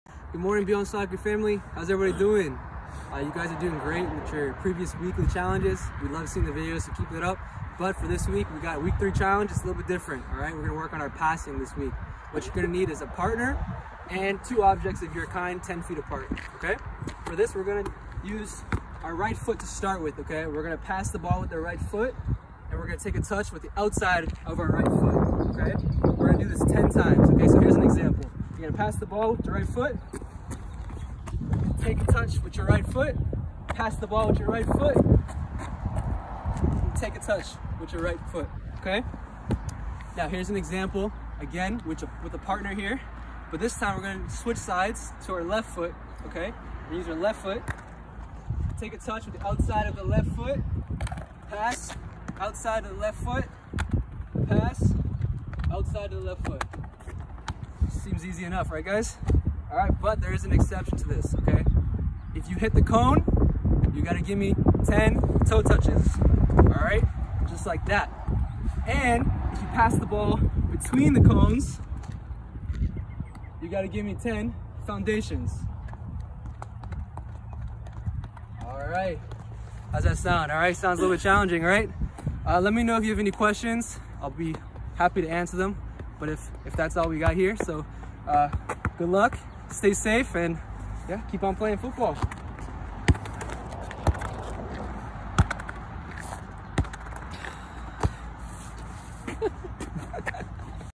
Week 3: At Home 'Beyond' Soccer Skillz Challenge: PASSING
Welcome to week three of our at home Soccer Skillz Challenge! We hope you've been practicing your skills from week one (trapping) and week two (turning)! This week we are building off of those skills to practice our passing. Passing is a very important soccer skill so this week we challenge you to practice your passing with touches! Grab a partner (if you can) or do it against a wall and get out there and give it a try!